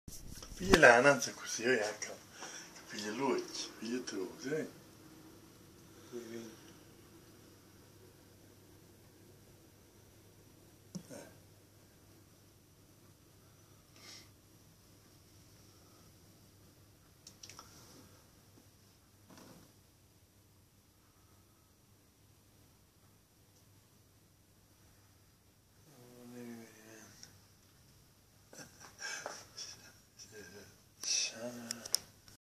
Baby shark found into squids belly!
If anyone knows what kind of shark it is, post a comment thank you.
The voices you hear are from my father and me, unfortunately he called me when he had already disposed of the squid in little rings ready to fry, so I couldn't show the actual squid.